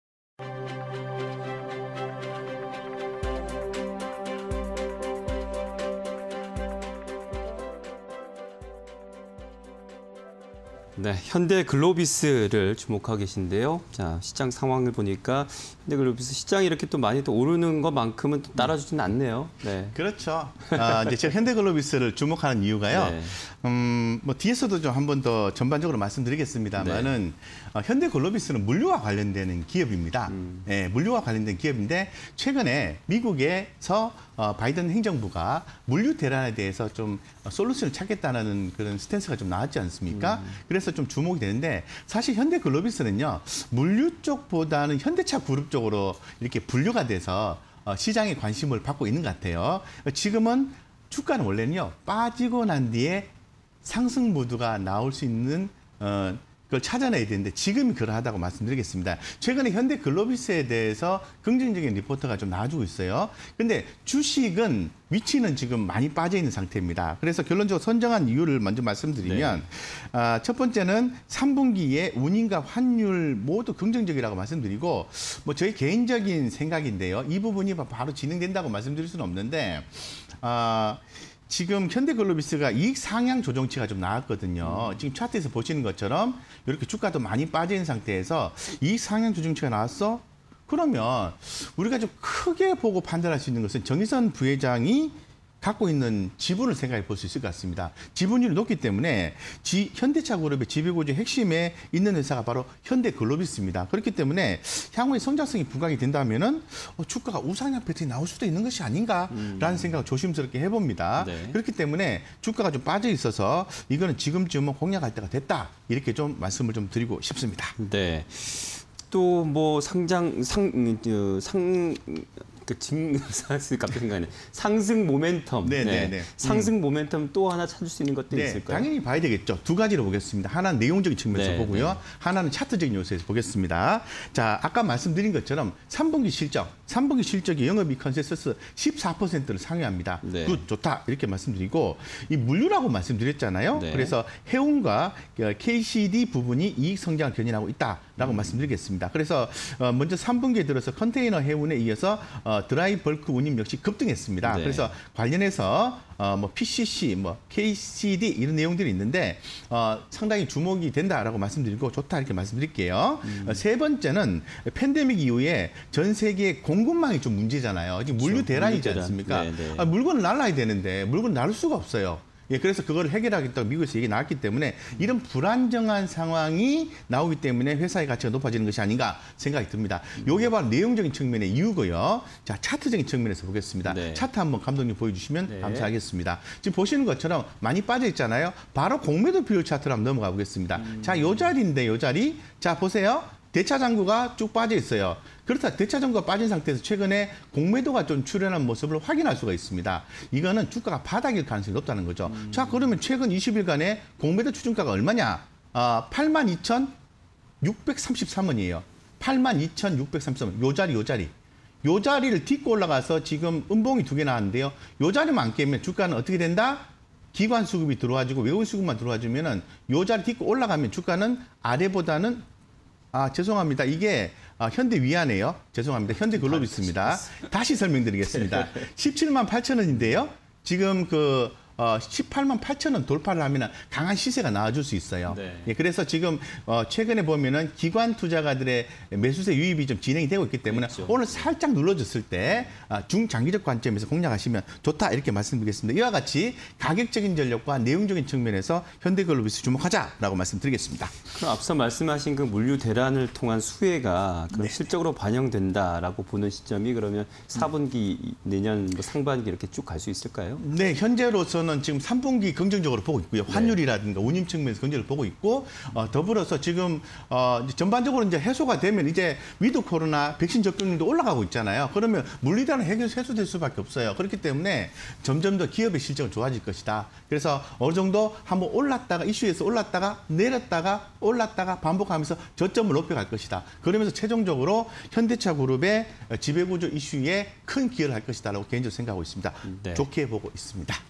현대글로비스(086280)... 3분기 운임ㆍ환율 모두 긍정적 전망_테마IN이슈 (20211015)
현대글로비스(086280)
- 3분기 운임ㆍ환율 모두 긍정적 전망
- 이익 증가 전망
- 신사업의 성장성 부각 등 우상향 기대